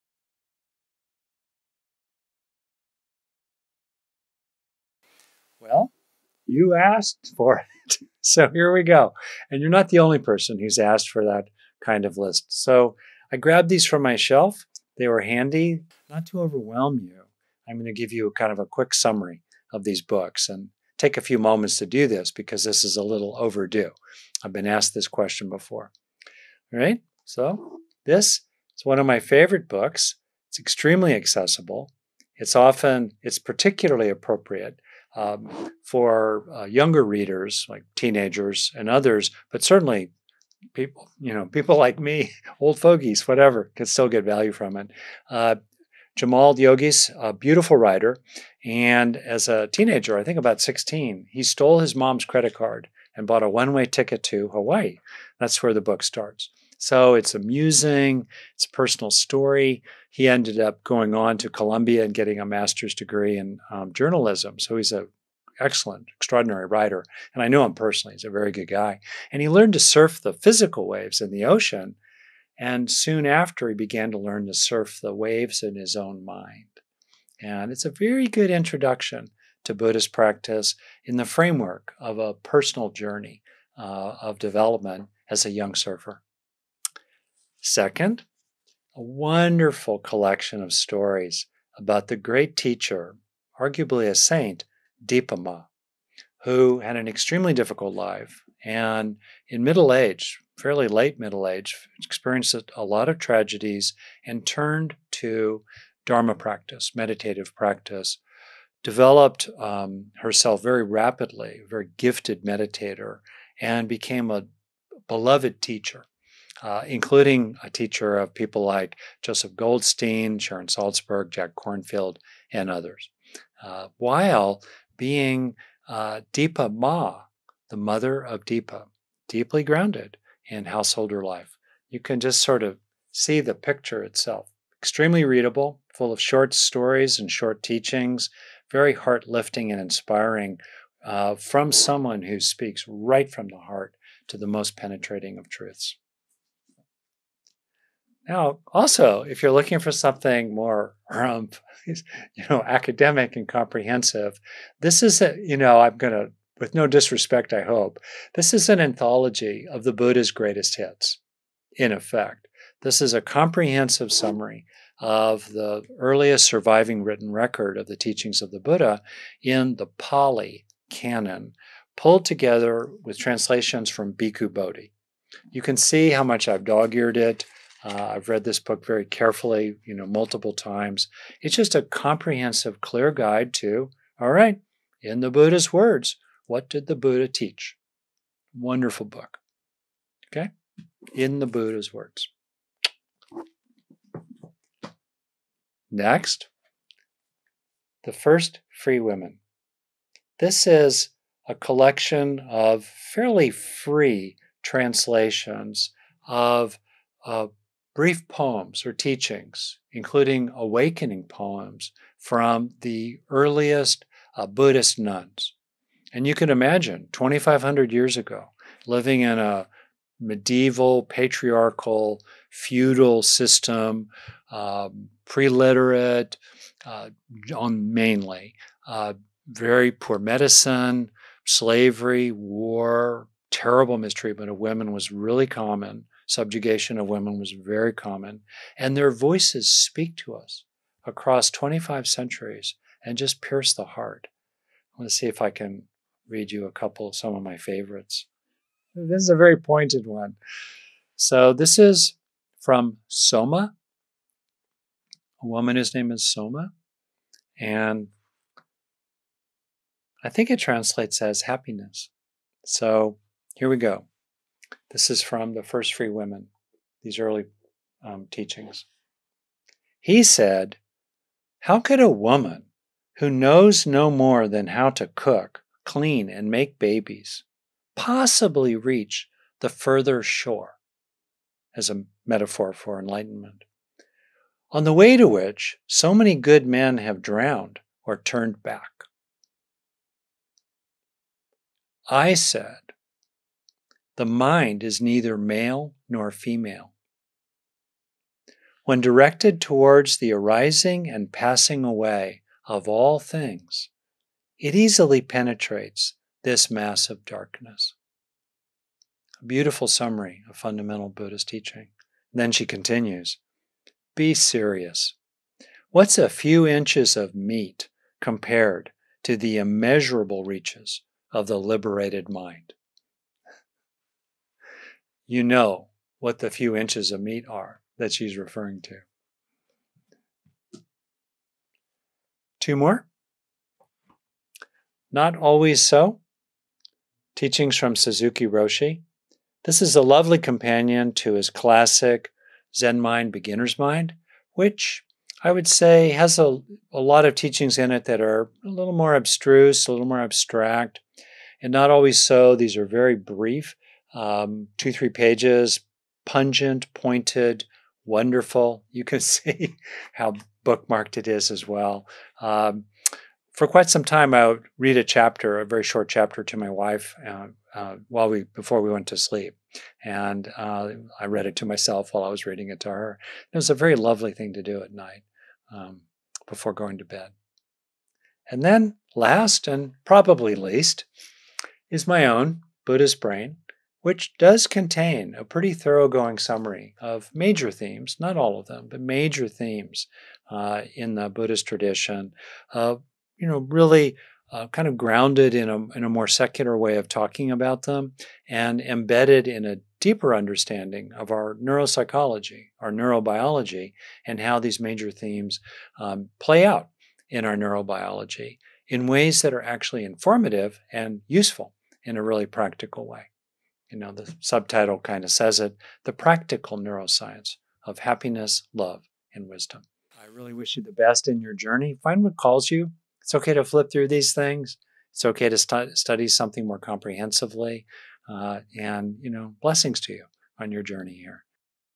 Can You Recommend a Few Books about the Buddha?
This question is from the informal pre-gathering Q&A from Rick's weekly Wednesday Meditation, which is free and open to anyone